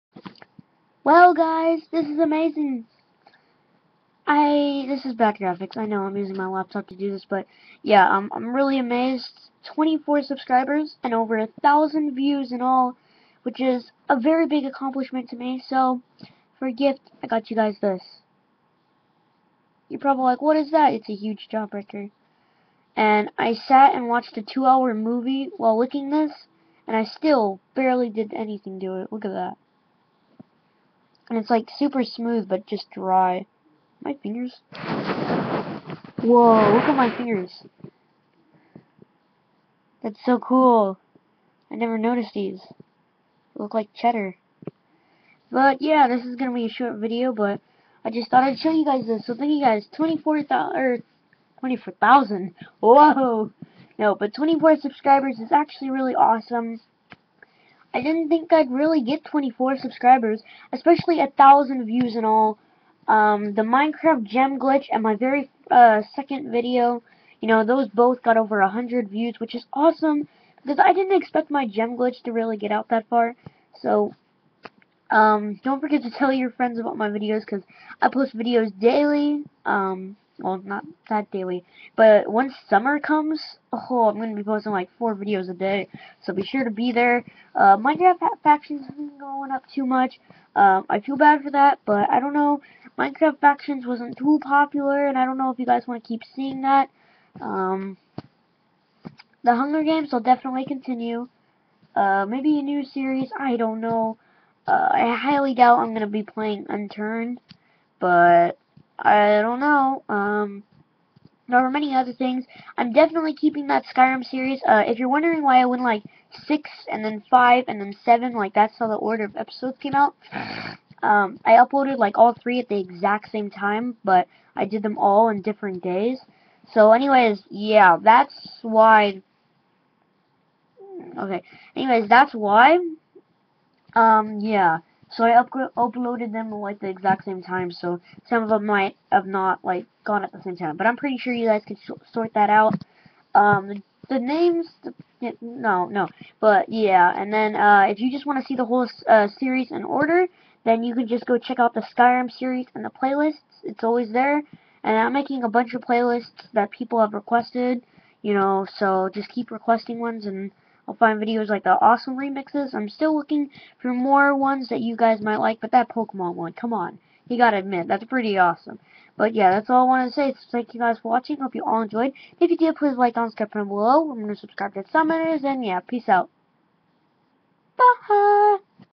24 Subscribers And Over 1000 Views? What?!!!
Well today i would just like to say WOW24 subscribers and over 1000 views all together now that is amazing i mean i put a hole in my roof not kid phadora style but ya know what i mean its just really awesome fore your guys support so Thank You all of you who watch my videos